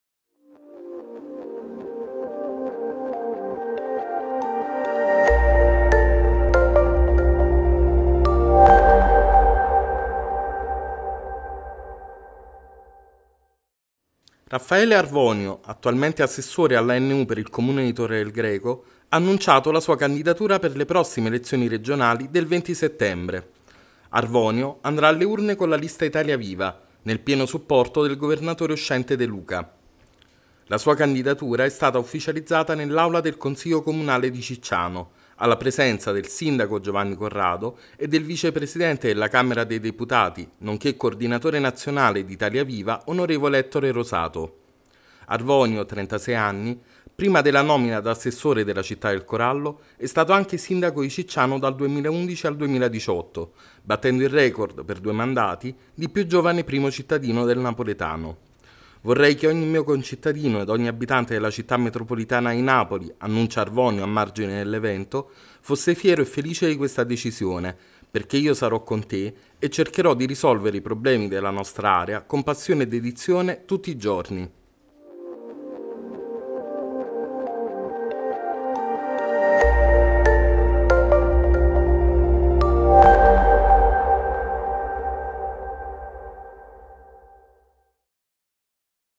Raffaele Arvonio candidato alle prossime regionali: "Pieno supporto a De Luca" - VIDEO
Raffaele Arvonio, attualmente assessore alla NU per il Comune di Torre del Greco, ha annunciato la sua candidatura per le prossime elezioni regionali del 20 settembre.

Arvonio andrà alle urne con la lista Italia Viva, nel pieno supporto del Governatore uscente De Luca.

La sua candidatura è stata ufficializzata nell’aula del Consiglio Comunale di Cicciano alla presenza del sindaco Giovanni Corrado e del Vice presidente della Camera dei Deputati, nonché coordinatore nazionale di Italia Viva, Onorevole Ettore Rosato.

Arvonio, 36 anni, prima della nomina ad assessore nella città del corallo, è stato anche sindaco di Cicciano dal 2011 al 2018, battendo il record, per due mandati, di più giovane primo cittadino del Napoletano.

“Vorrei che ogni mio concittadino ed ogni abitante della Città Metropolitana di Napoli  - annuncia Arvonio a margine dell’evento - fosse fiero e felice di questa decisione, perché io "sarò con te", e cercherò di risolvere i problemi della nostra area, con passione e dedizione, tutti i giorni”